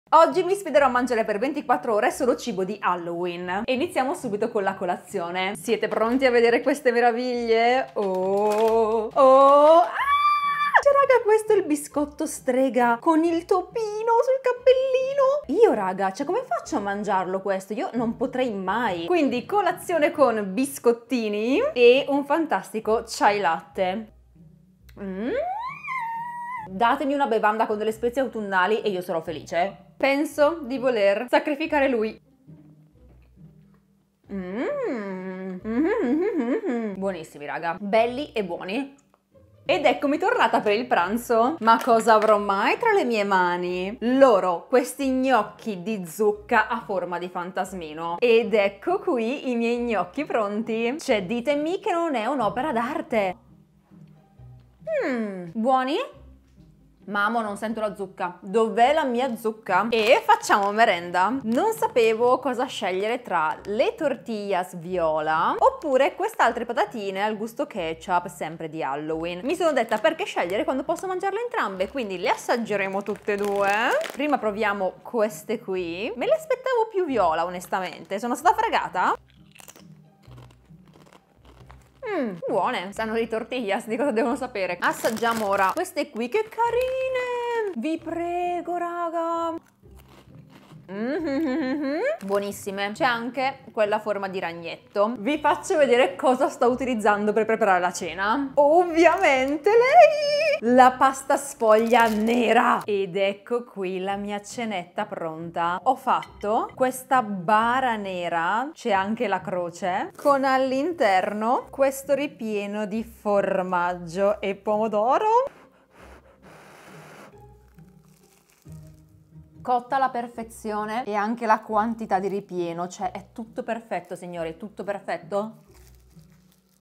MANGIO SOLO CIBO DI HALLOWEEN PER 24 ORE
attrezzatura utilizzata per il video:
- SONY A6300
- RØDE VideoMicro
- FOSITAN 20"X28" Softbox Kit 1600W 5500K